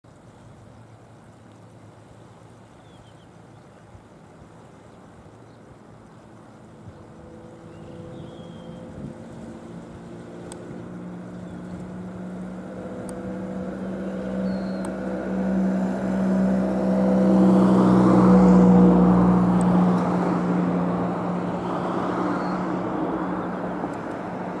５月２２日日曜日みなべ町堺グランド横からのアオリイカ釣りの様子
３台のライブカメラで海況等も調べることができるホームページは